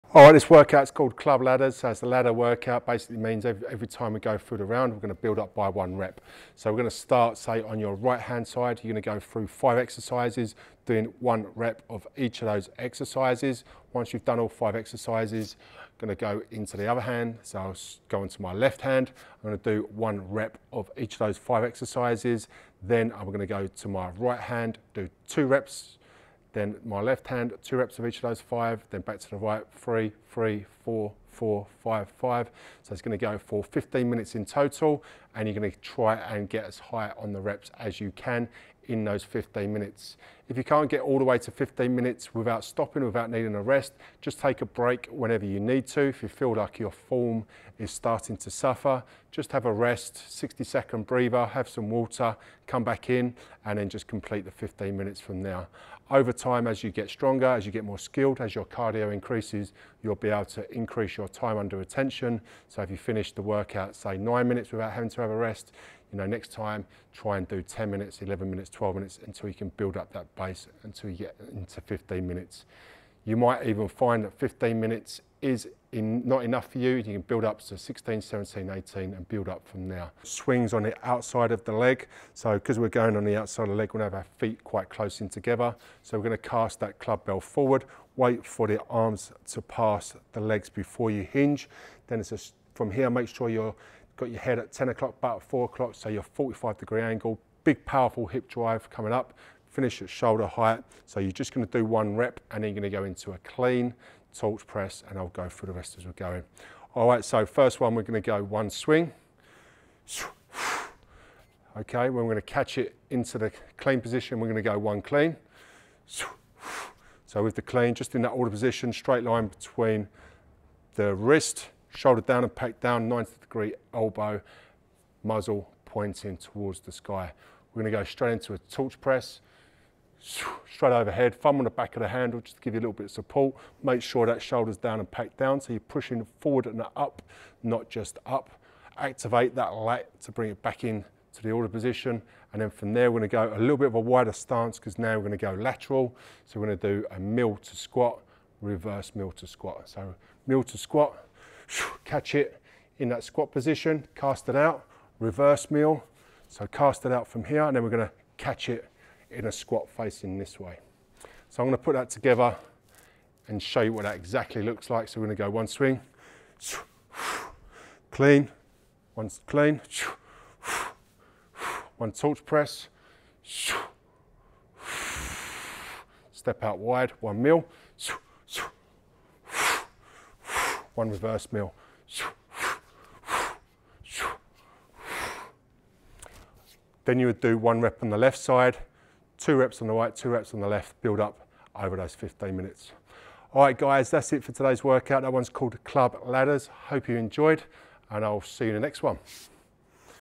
Steel Club Workout #3 - Clubbell Ladders
Workout Instructions

Perform 1 rep of each exercise, swap hands and perform 1 rep on the other side. Then perform 2 reps of each of each exercise, 3 reps etc. Get as high on the reps as you can in 15-minutes, if possible try not to stop. If you need to, take a quick break and resume your set.

Exercises:

* Swing
* Clean
* Torch press
* Mill to squat
* Reverse mills to squat